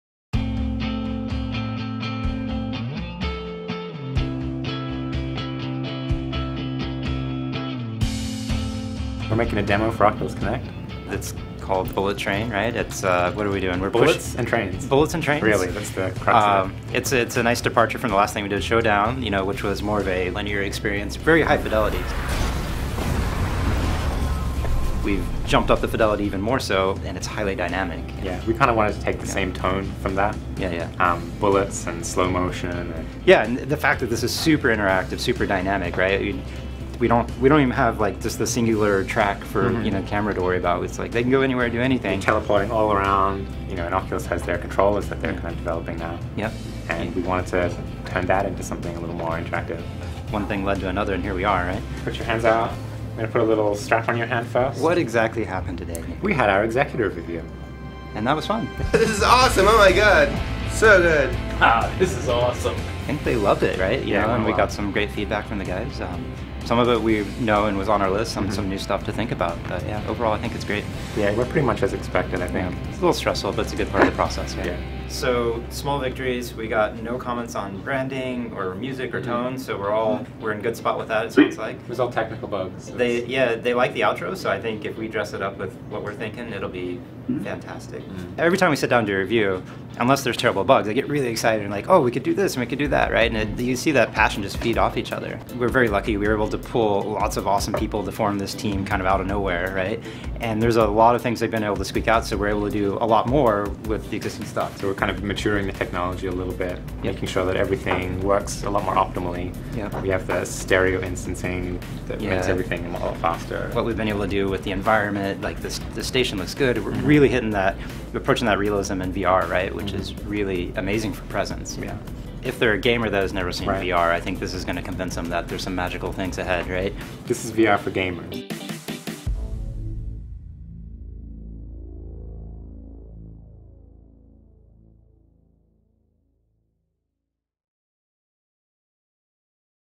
Making of Bullet Train: The Official Unreal Engine 4 VR Demo
Step behind the scenes with Epic Games in the weeks leading up to Oculus Connect 2 as Bullet Train takes shape and becomes the studio's most ambitious VR experience to date.

Add us on YouTube Gaming!